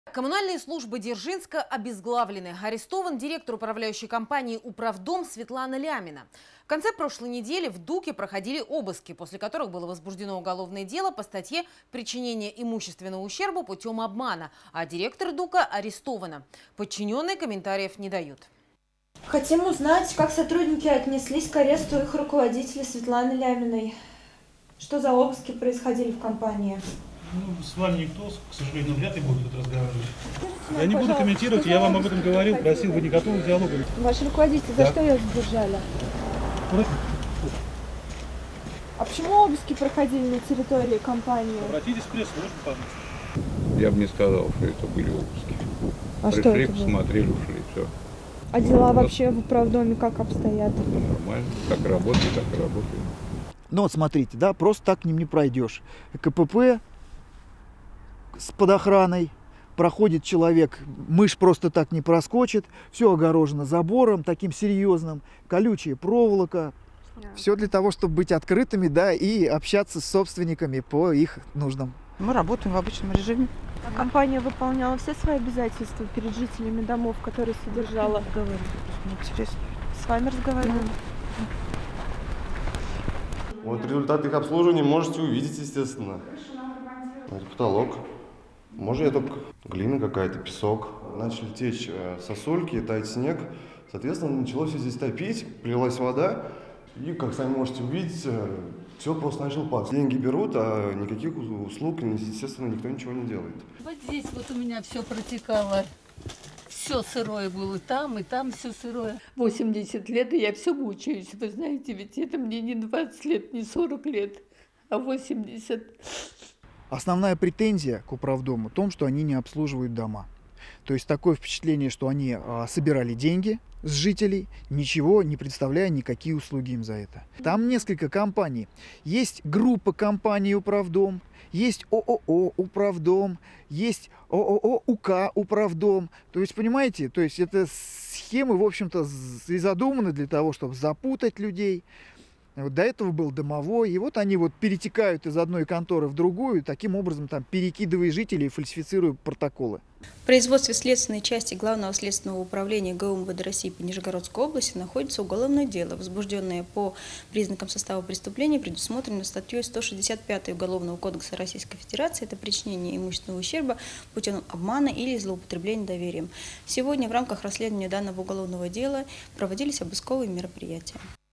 Директора главной коммунальной компании Дзержинска арестовали за обман жителей
Коммунальные службы Дзержинска обезглавлены - арестован директор ГК "Управдом" Светлана Лямина. В конце прошлой недели в ДУК проходили обыски, после которых было возбуждено уголовное дело по статье "причинение имущественного ущерба путем обмана", а директор ДУКа арестована. Подчиненные комментариев не дают.